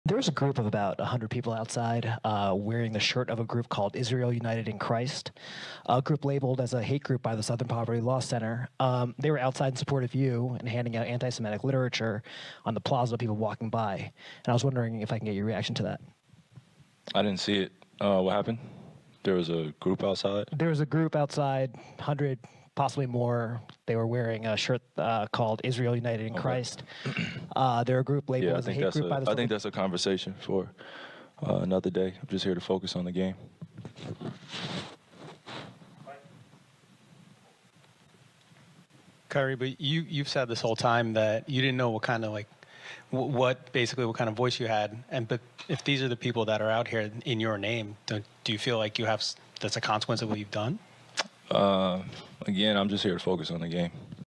Kyrie Irving on Hebrew Israelites showing up in support at Barclays Center
credit to @MobAlphaNation
credit to @Back-Court
credit to @GlockSwitchGTA